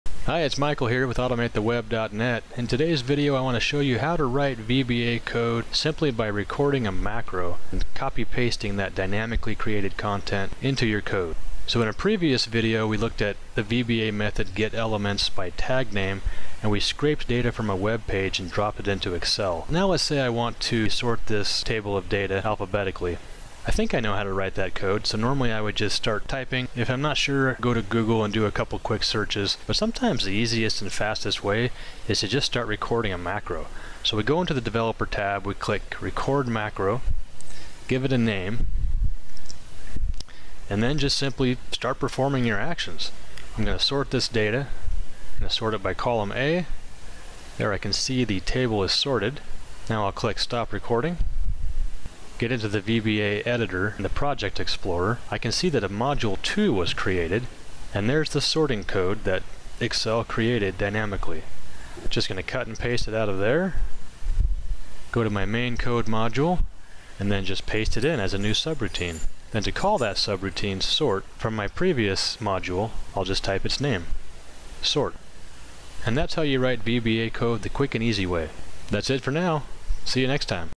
How to write VBA code by recording macro
Sometimes the fastest, easiest way to write tricky Excel VBA code is by recording a macro, then copying its dynamically-generated code.